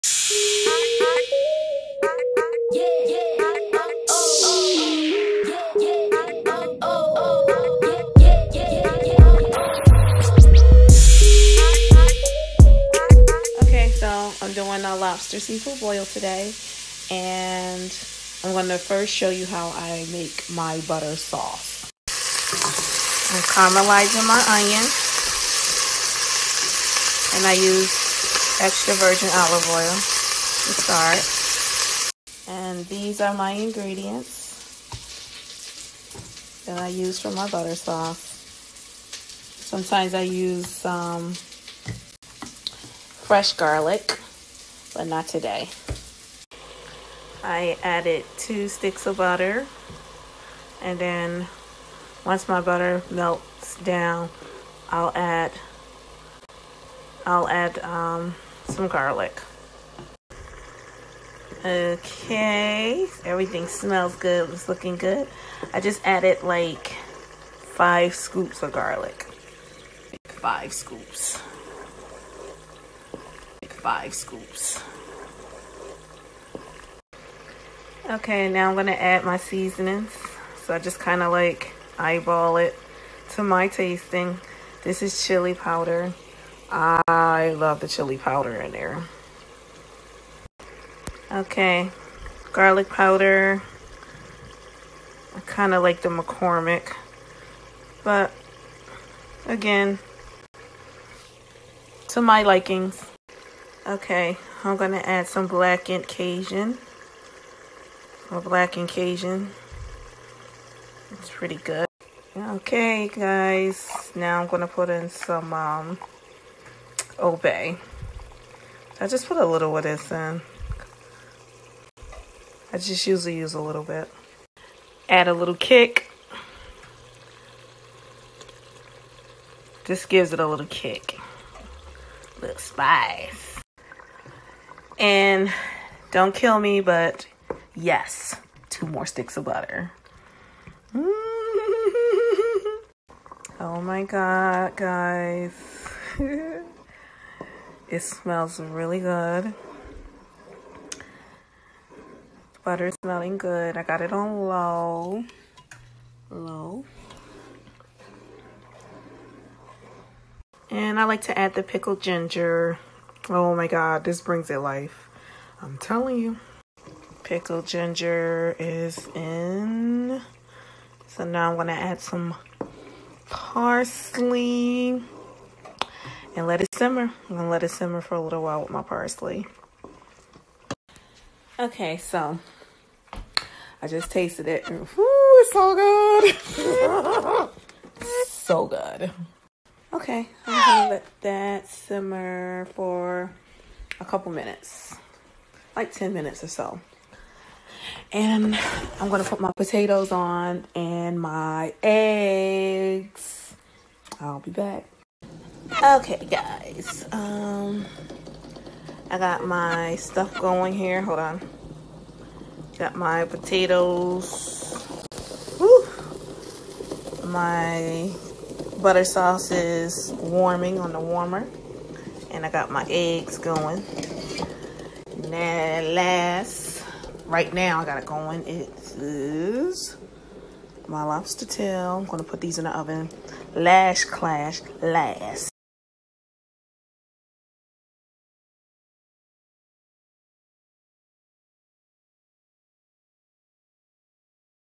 How I make my BLOVE INSPIRED BUTTER SAUCE recipe yaaaasss!!!!
Hey ya’ll!!! In this video I am making one of my Blove inspired butter sauce recipes for my lobster tails.